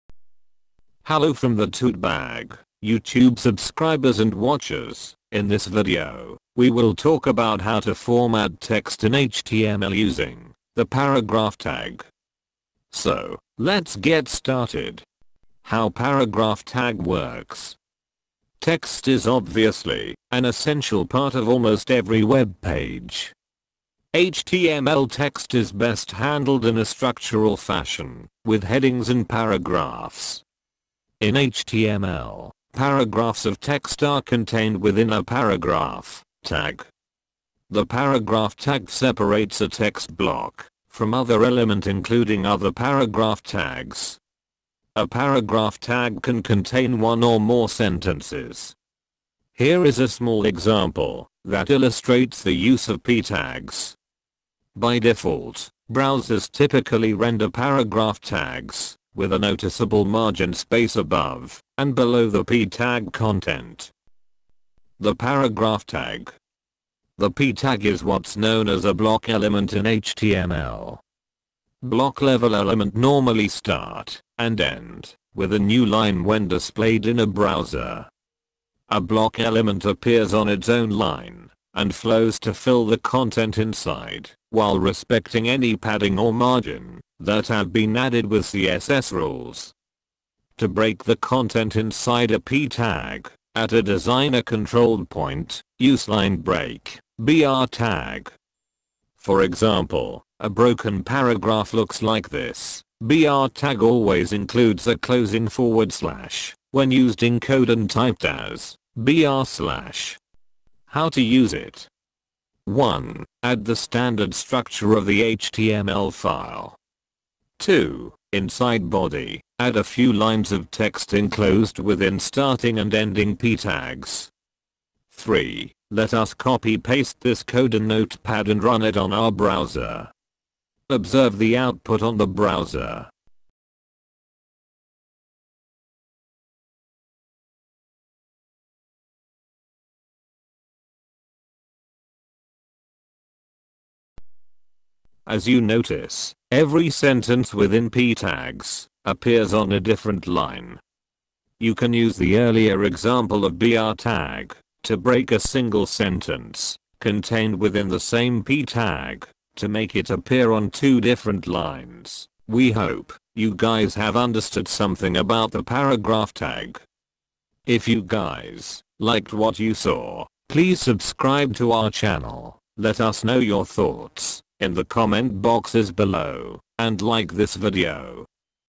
No5: HTML paragraphs (p) tag: HTML5 Tutorial For Beginners [HD 1080p]
This HTML5 Tutorial For Beginners video shows how to use the HTML paragraph tag in your code to format text into paragraphs. Each paragraph begins on a new line when displayed in the browser and it can be broken into separate lines using the line-break or BR tag.